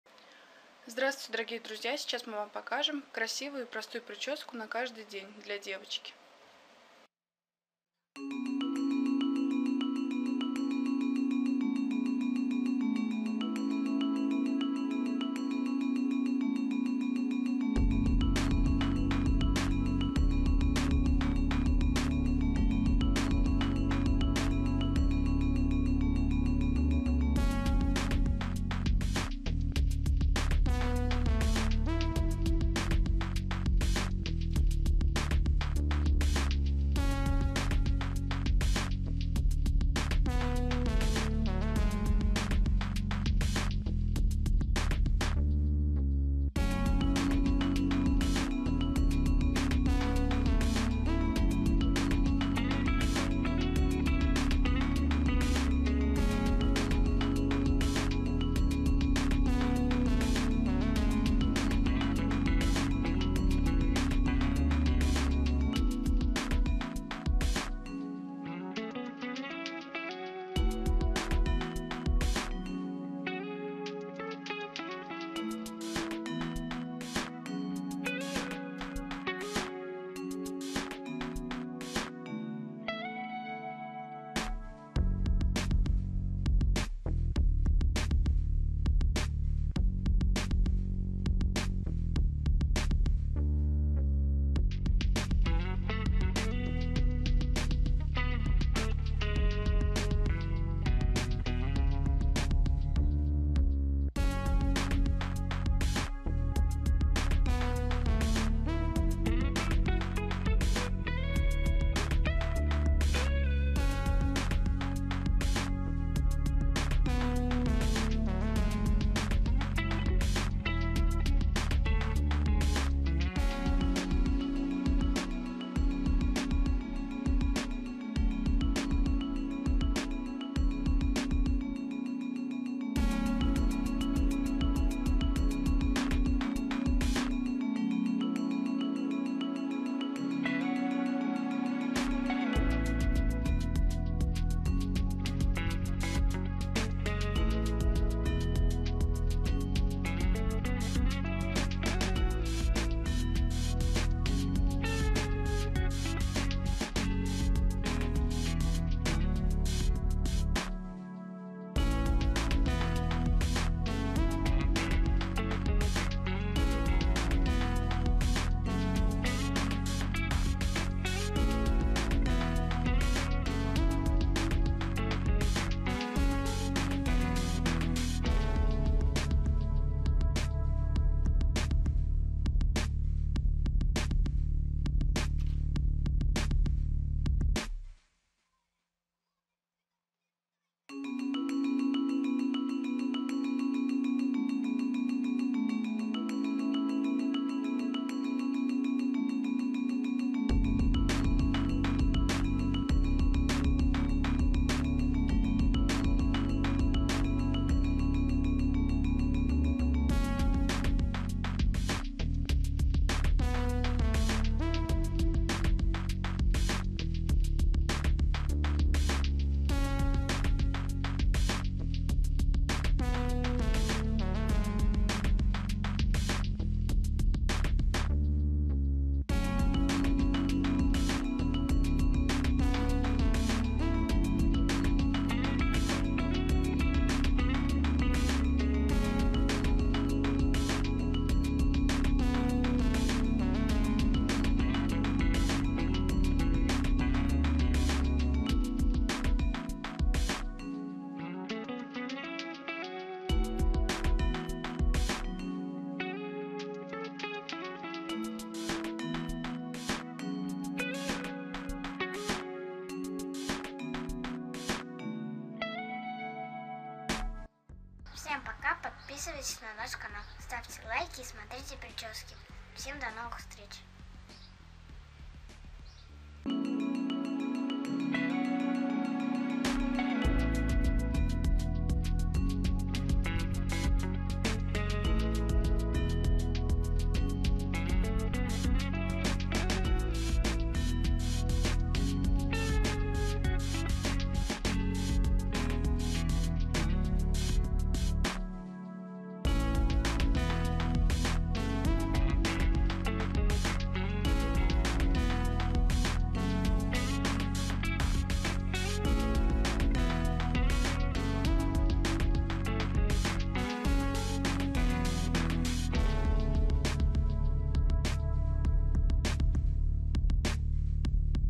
♥ОЧЕНЬ ПРОСТАЯ ПРИЧЕСКА ДЛЯ ДЕВОЧКИ НА КАЖДЫЙ ДЕНЬ/ПРИЧЕСКА С ПОМОЩЬЮ РЕЗИНОК♥
♥В этом видео мы покажем Вам очень простую прическу для девочки на каждый день с помощью резинок!

Будьте в курсе всех тенденций!